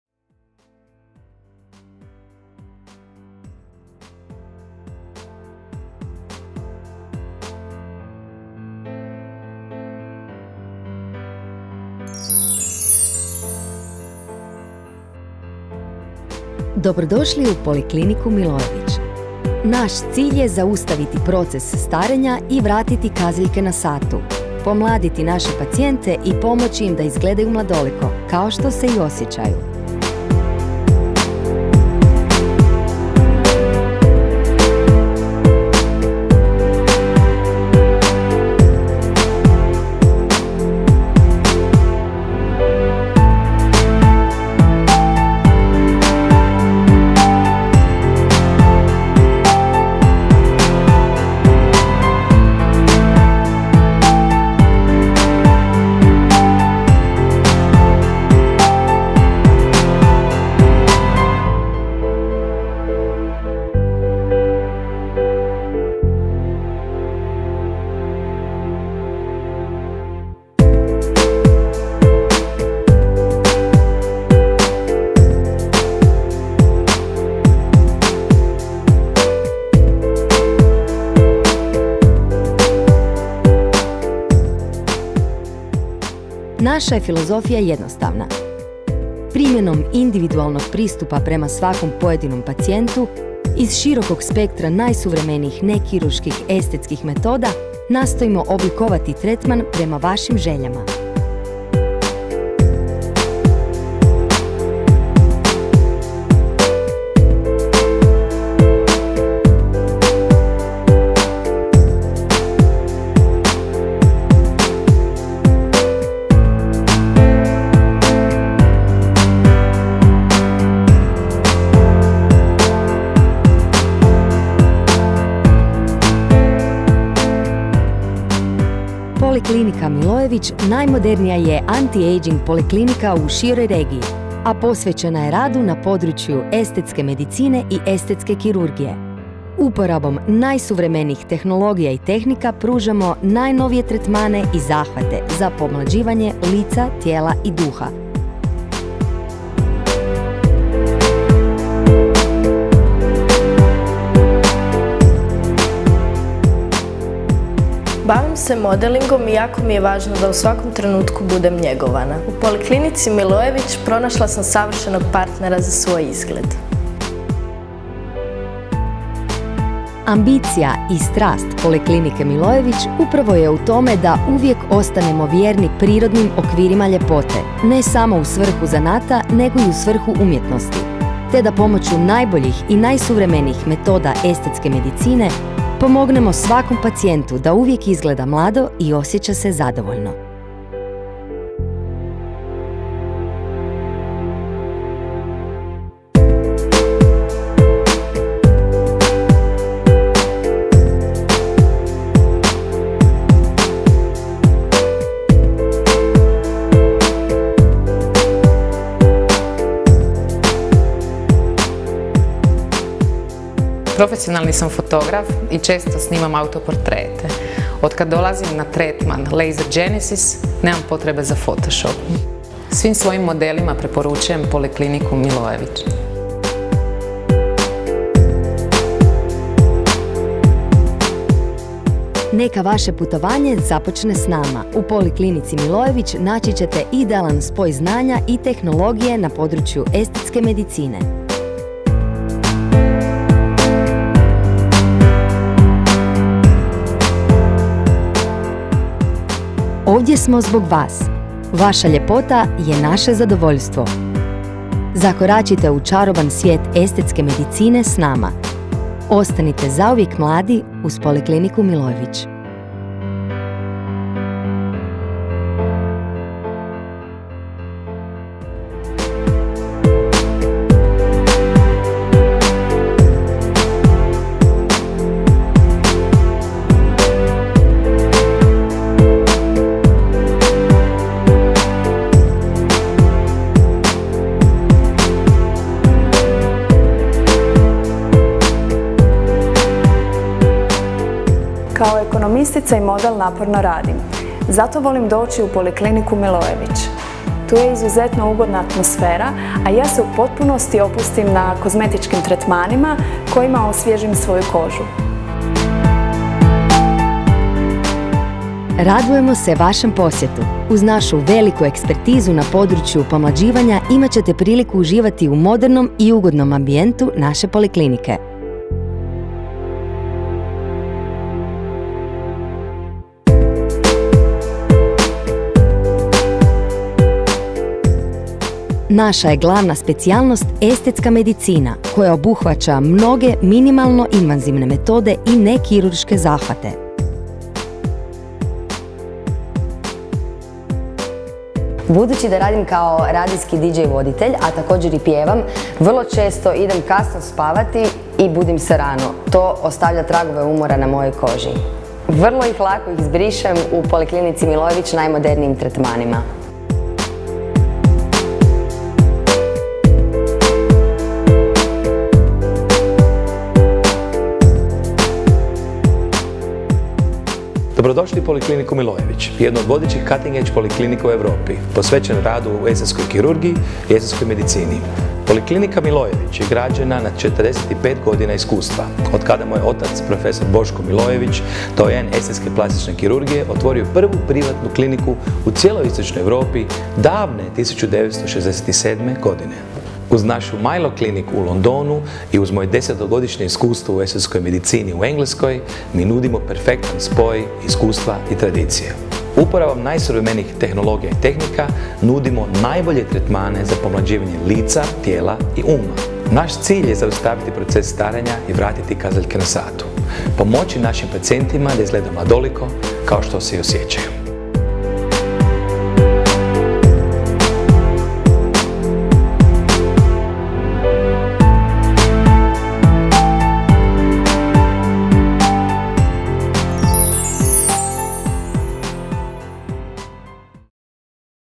Poliklinika Milojevic za estetsku medicinu i kirurgiju, Promo Video HD
Povodom proslave 2. godišnjice Poliklinike Milojević, snimili smo ovaj fantastičan i informativan promo video o našoj Poliklinici, o njenoj filozofiji,predivnom izgledu same klinike te tretmanima i operacijama koje nudimo. Uz botox, dermalne filere, lasere i kemijske pilinge iz svijeta nekirurške estetike, pa sve do operacija kao Vaser Lipo, ultrazvučna liposukcija, oblikovanje tijela, uklanjanje madeža i blefaroplastike, Poliklinika Milojević je vodeća cutting edge estetska Poliklinika u Europi. Uz vodstvo Dr Nikole Milojevića, nudimo perfektan spoj iskustva, tradicije, znanja i najmodernije tehnologije. Zavirite u naš svijet!